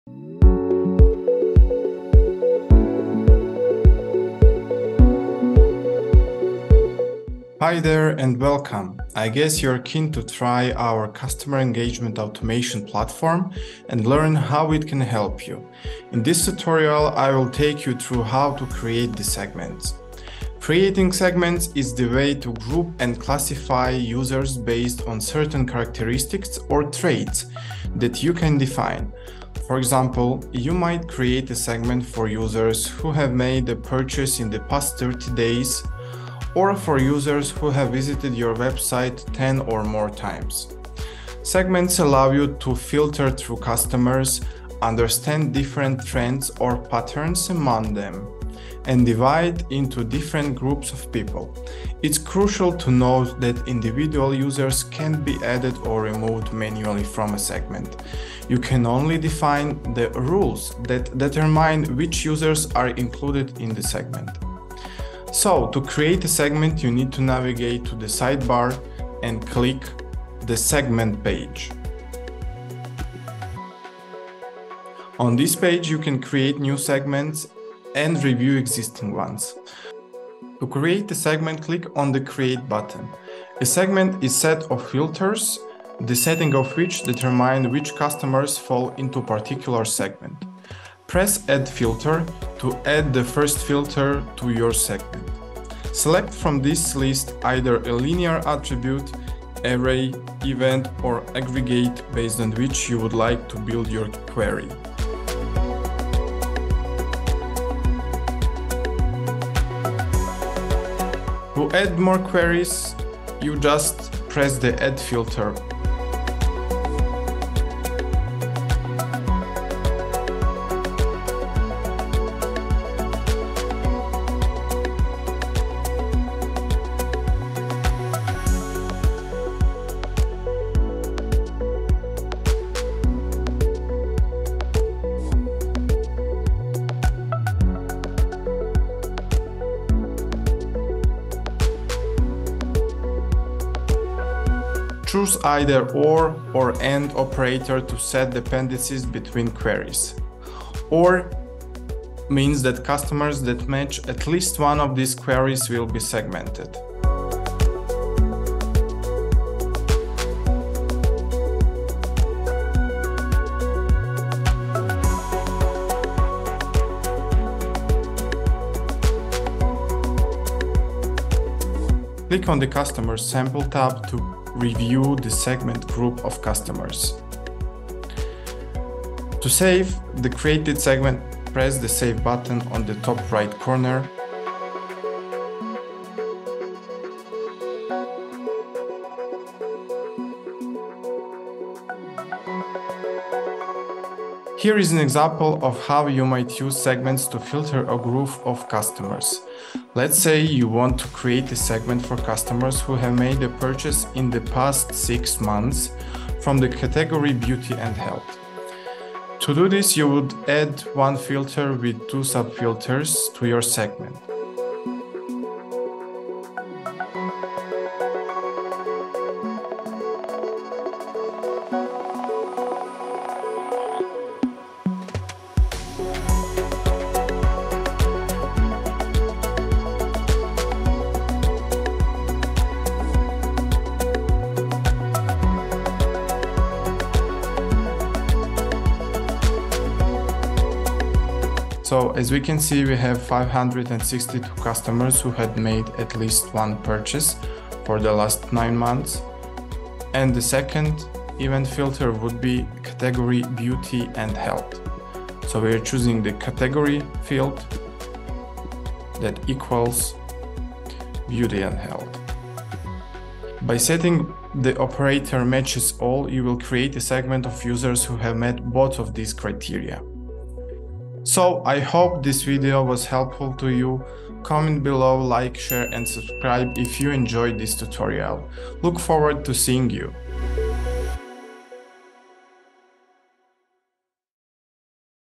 How to work with the audiences: Retainlys’ segmentation capabilities
Discover the power of customer segmentation with Retainly!
Watch a full tutorial on easily grouping and classifying customers based on their interests, behaviors, and purchases.
Learn how to create segments, define filters and queries, preview segmented groups of customers, and analyze customer trends.
Unlock the potential for creating impactful campaigns with Retainly's seamless customer engagement automation platform.